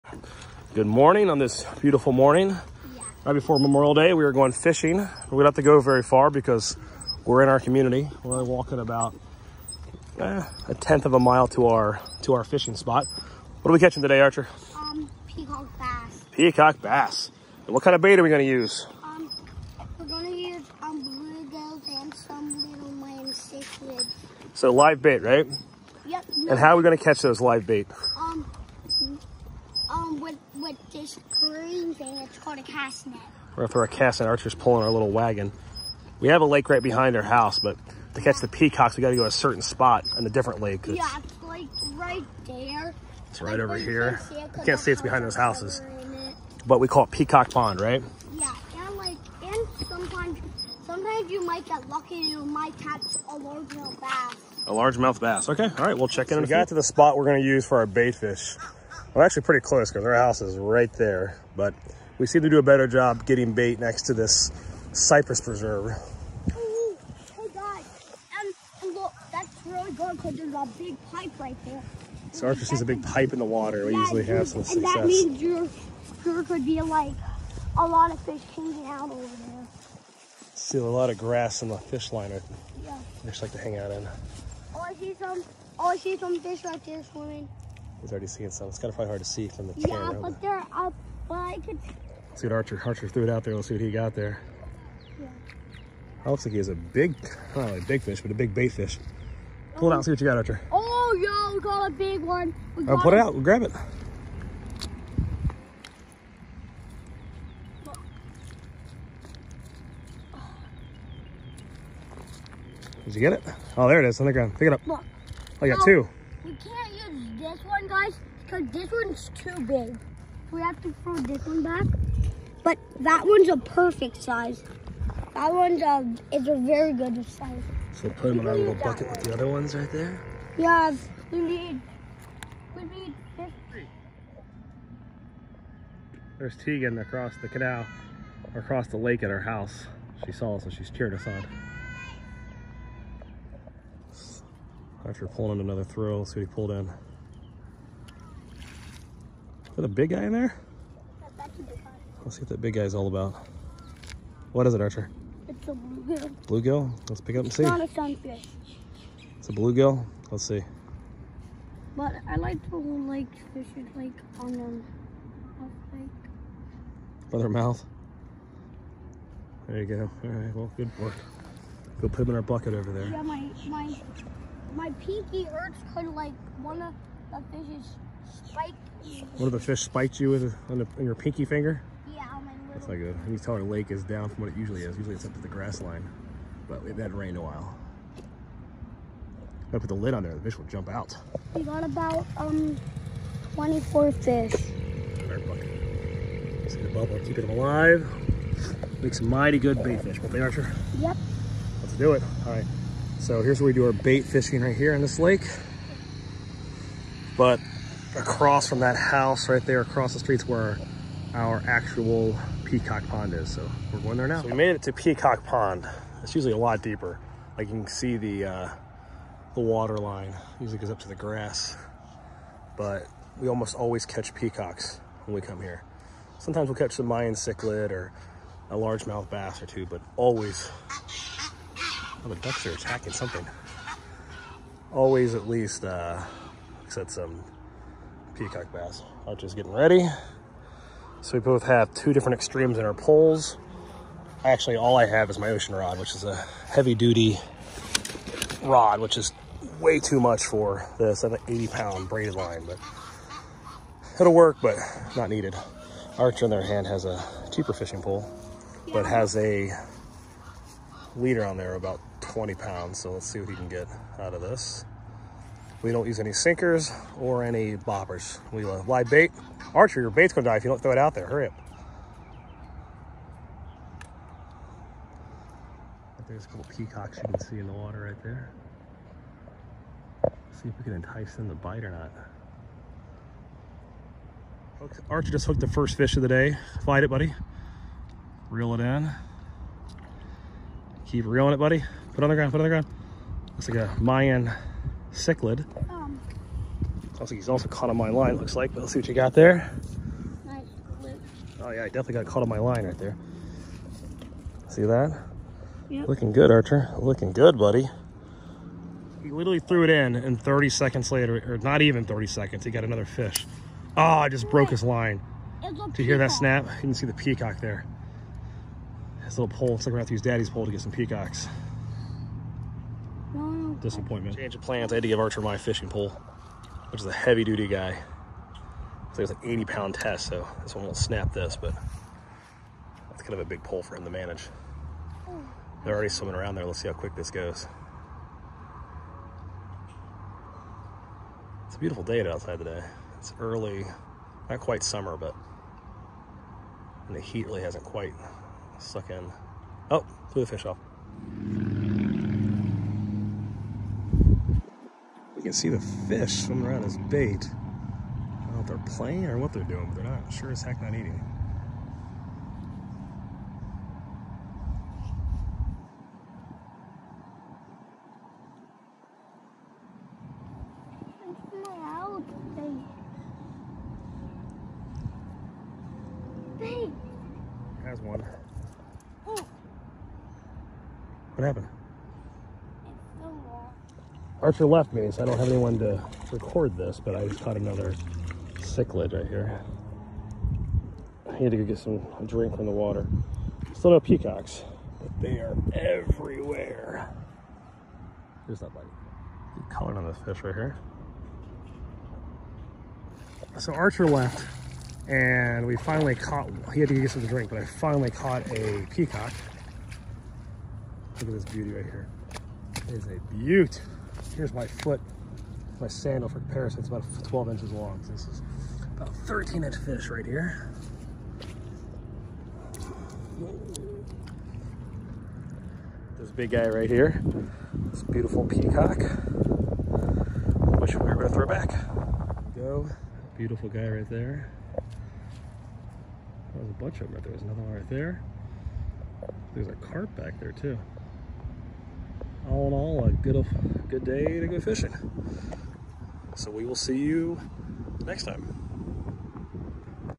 Peacock fishing with Daddy and Archer
Archer and Daddy went fishing in the lake by their home. It’s called “Peacock Pond” because it is crawling with peacock bass.